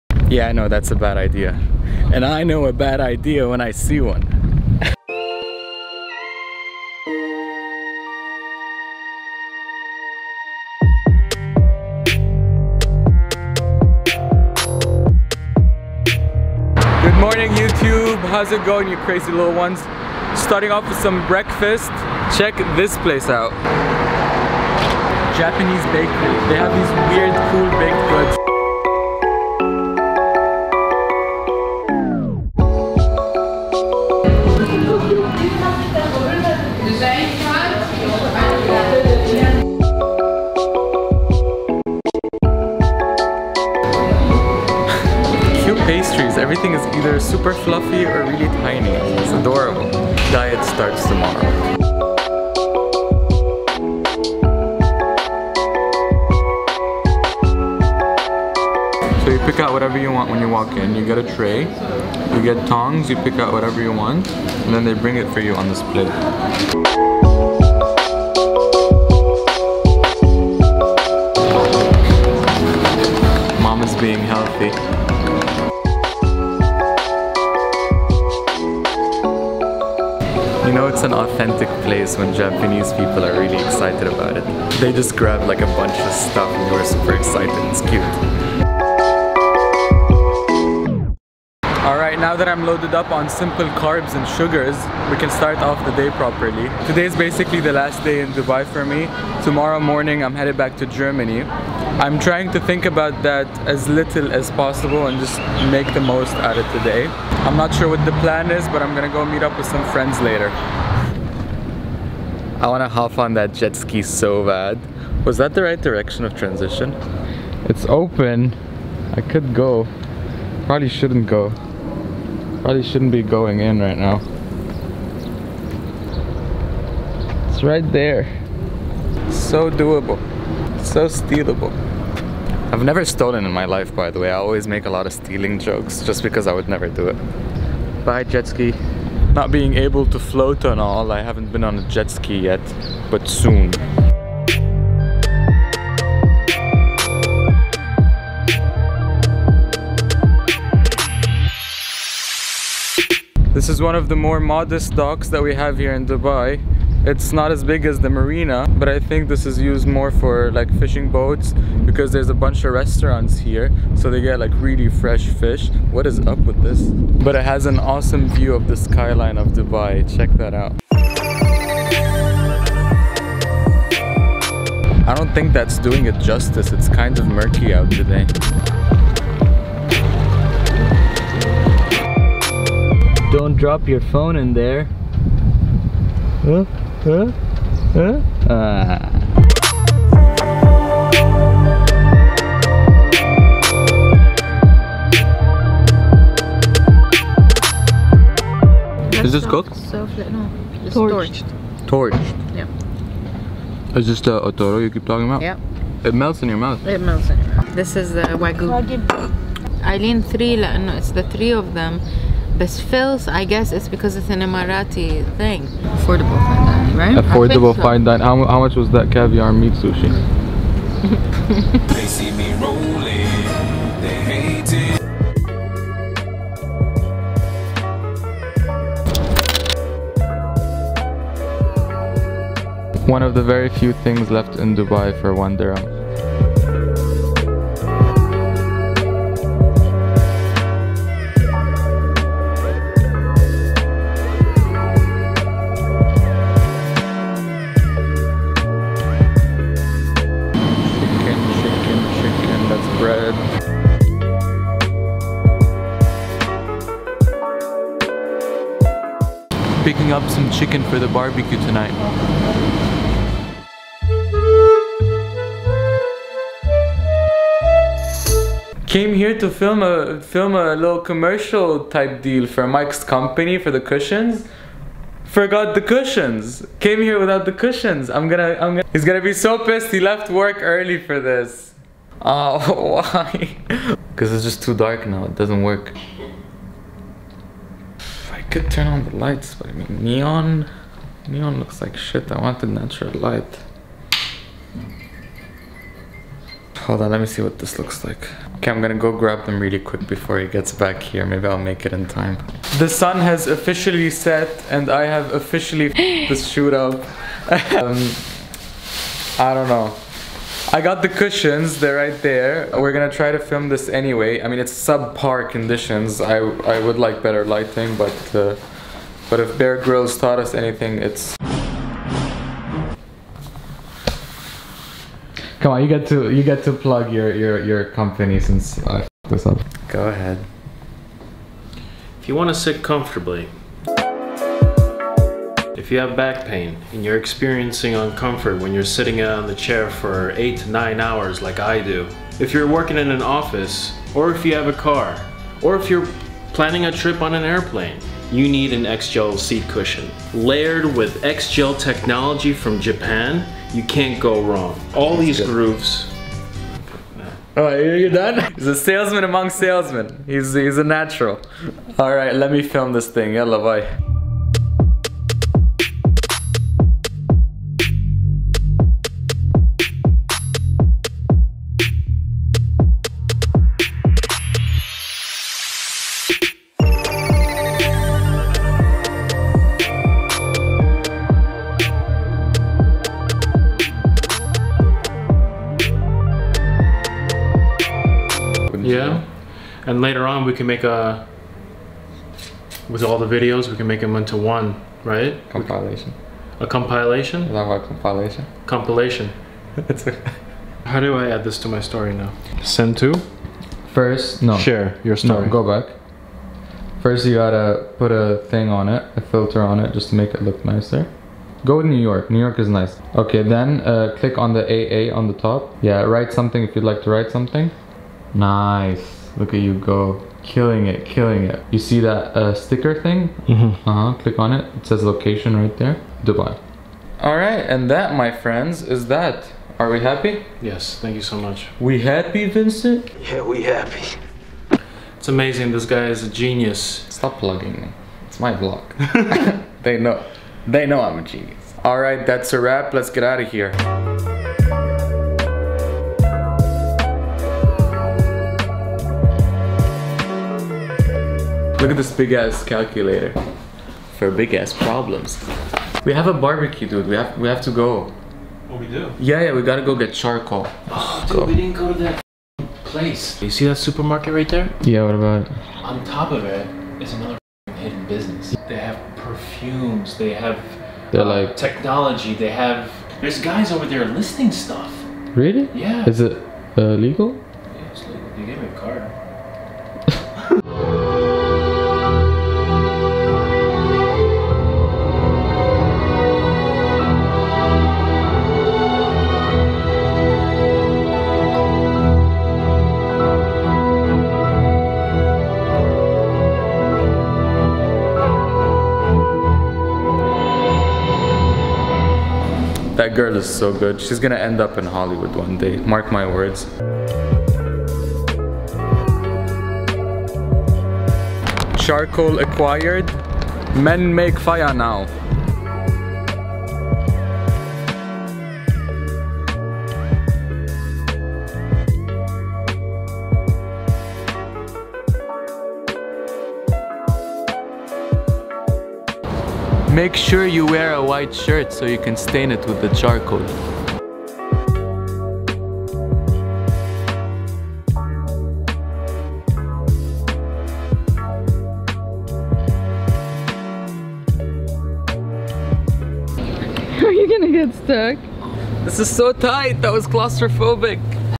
i f***ed up
it was a fun and eventful last day in dubai before i have to go back to uni, it always sucks to leave home. But i'll be back soon enough.

Mike's company: (the cushions are great if you have any back or neck pain check em out)